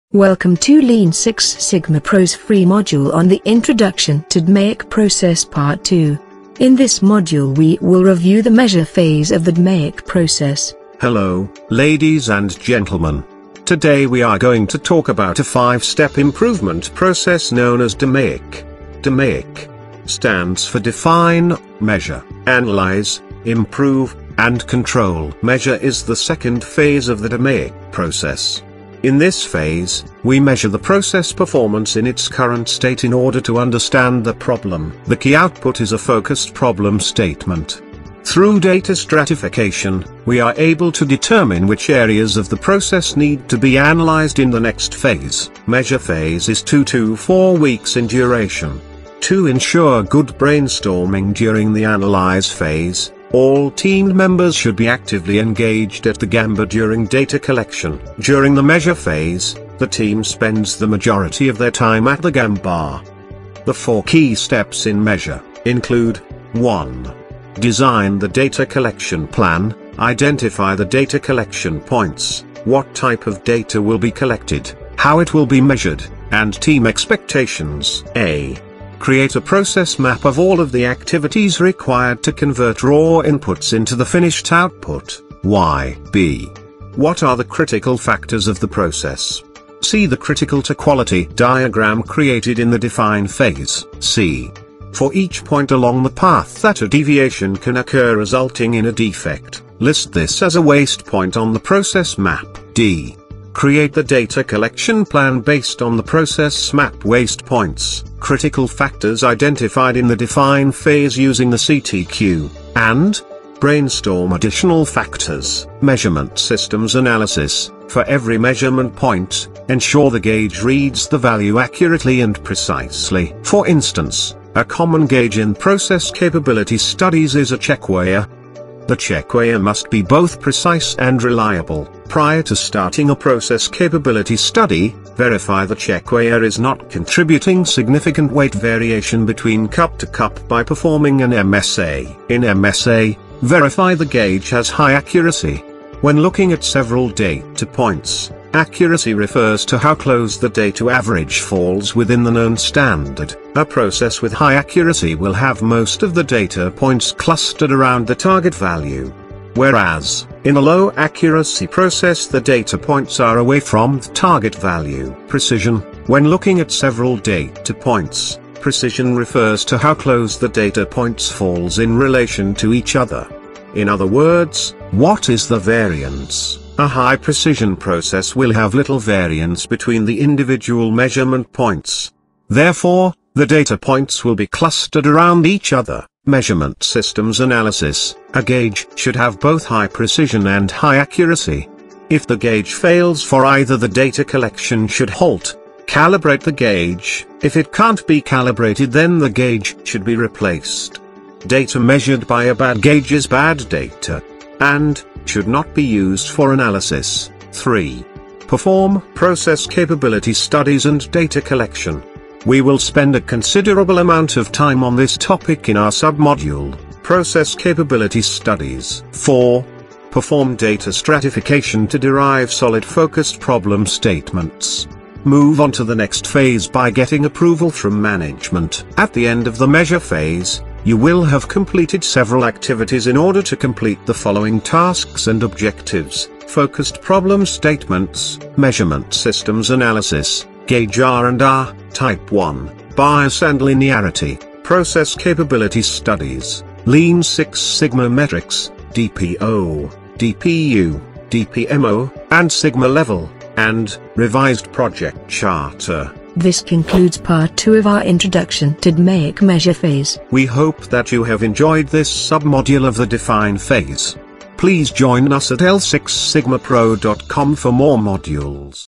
Introduction to DMAIC Part 2 Measure Phase v1.0
An introduction to the Measure phase of the DMAIC process. We divide the measure phase into 4 basic steps. This is a precursor to the Measure Phase full length module. Lean Six Sigma Training Modules from Lean Six Sigma Pro, L.L.C. A company dedicated to offering high-quality Free Lean Six Sigma Training.